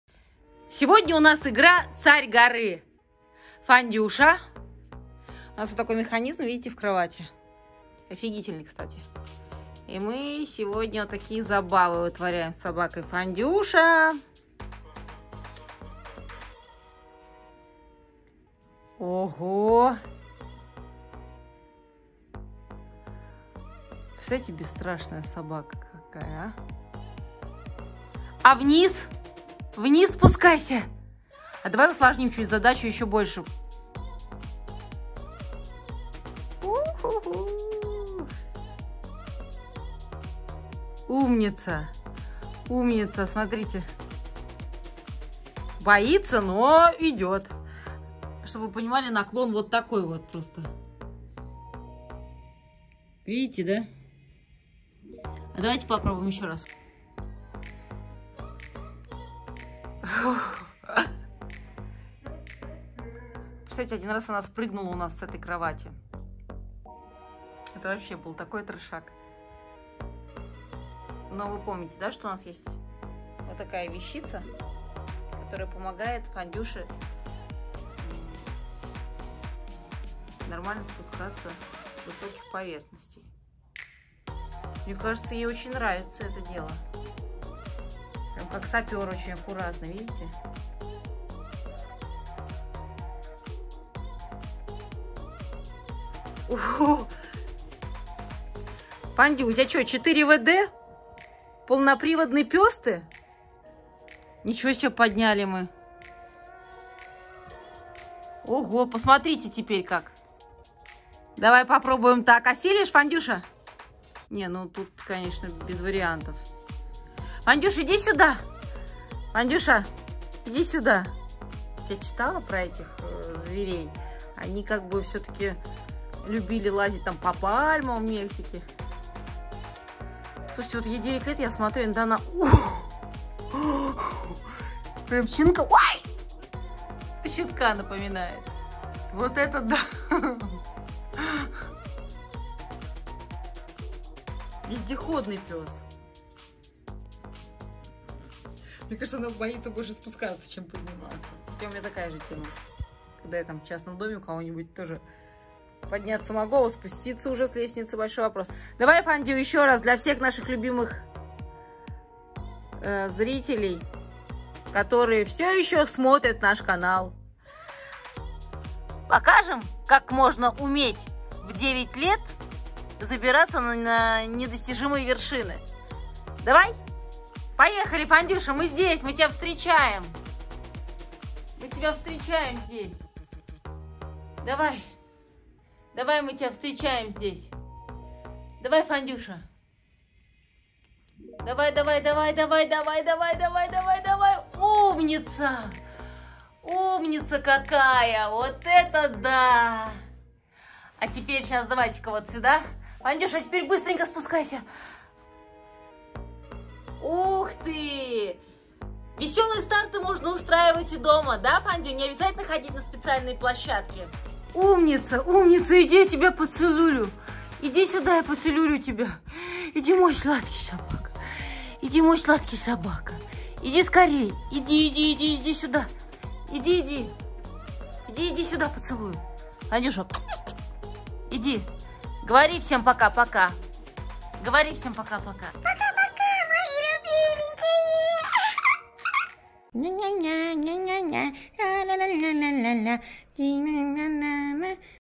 Смертельный трюк! Собака скалолаз покоряет вершины!
Когда нечего делать, можно придумать себе занятие из подручных средств. Например превратить обычную кровать в высокую гору и забраться на нее!